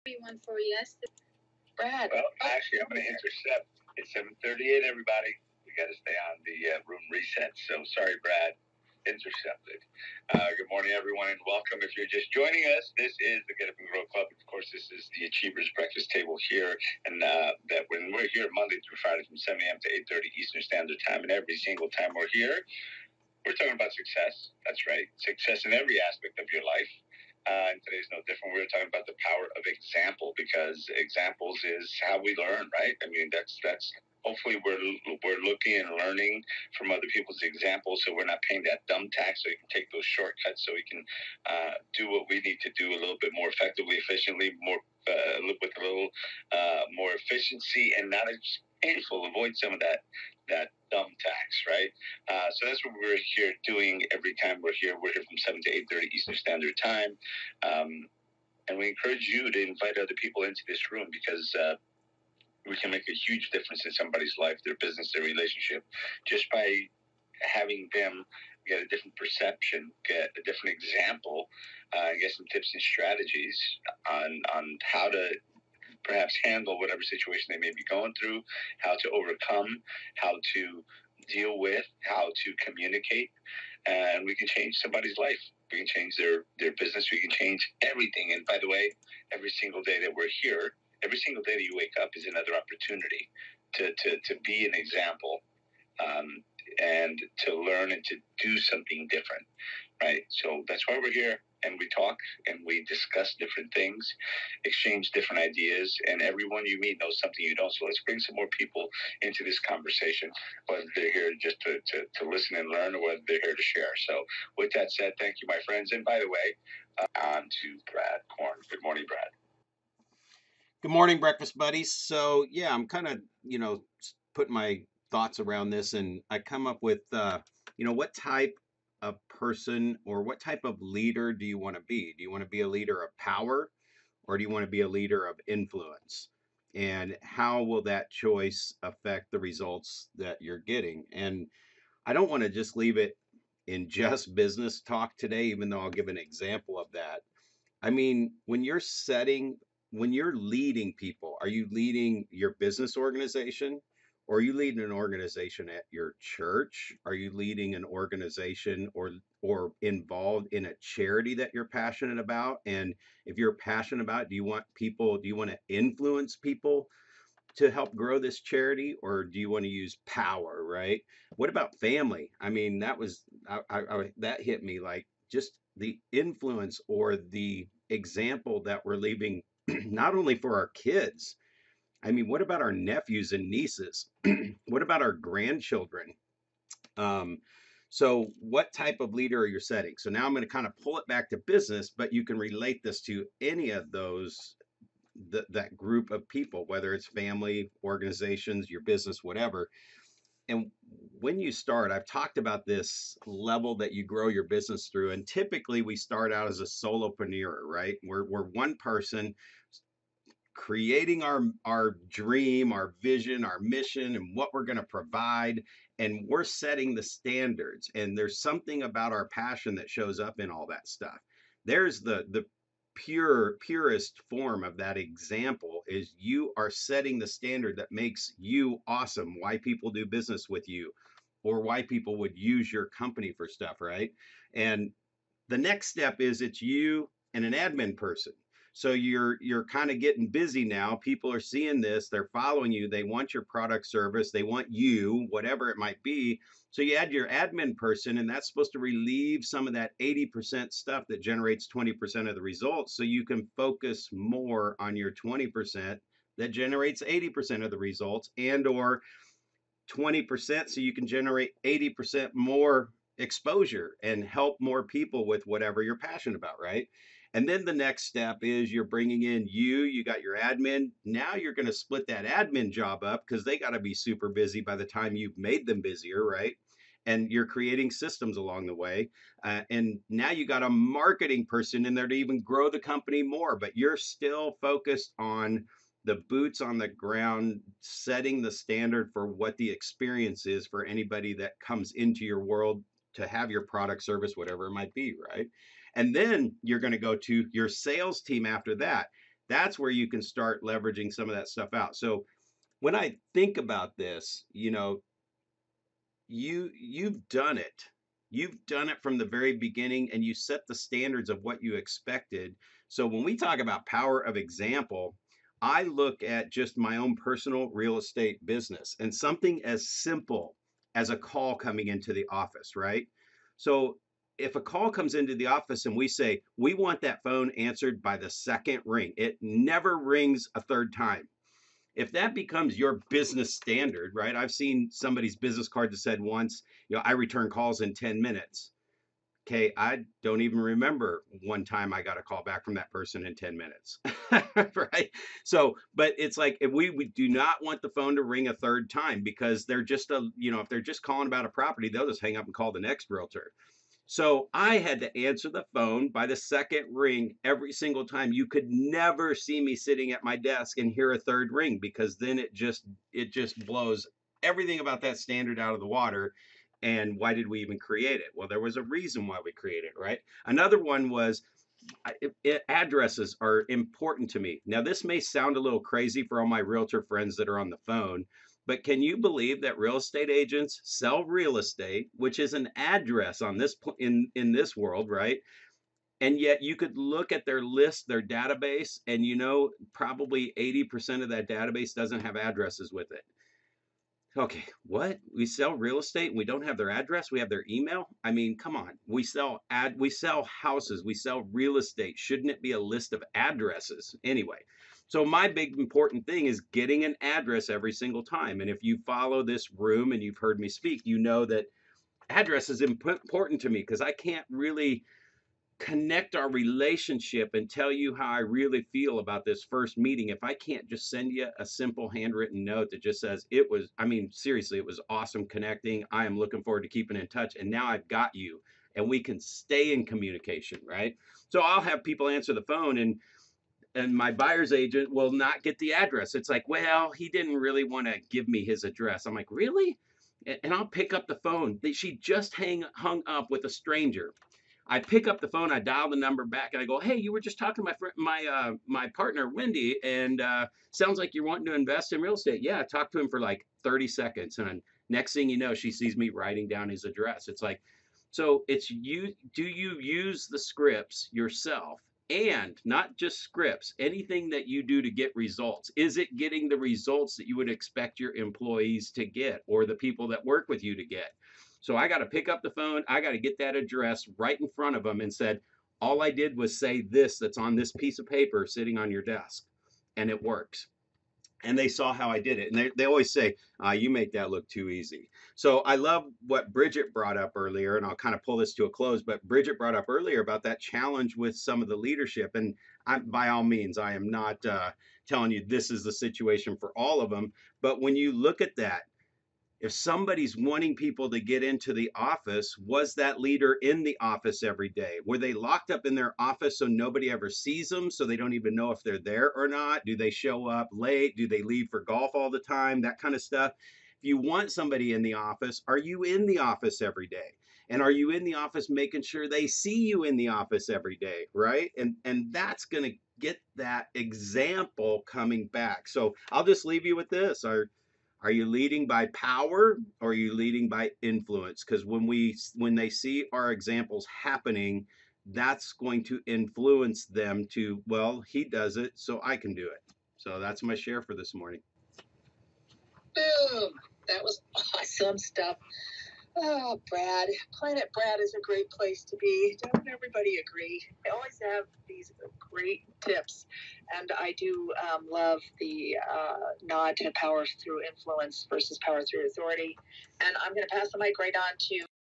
Power of Example - What Leader are you?
Do you want to lead by "POWER" or Influence? Do you do the things you expect others to do and more importantly, do you get the results you expect them to get?